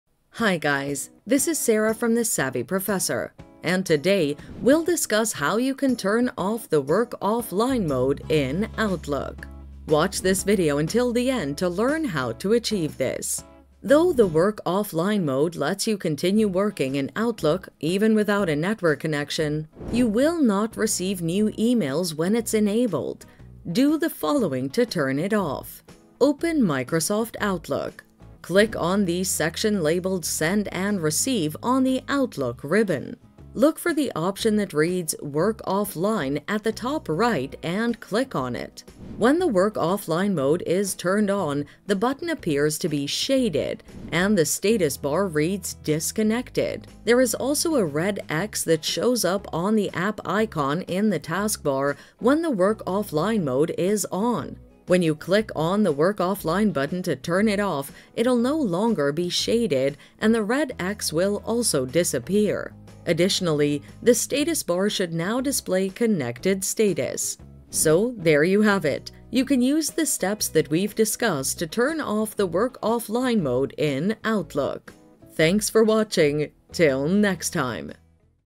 How to Turn Off Work Offline in Outlook (Switch from Working Offline to Online)
How to Turn Off Work Offline in Outlook (Switch from Working Online to Offline).In this tutorial, you will learn how to turn off work offline in Outlook.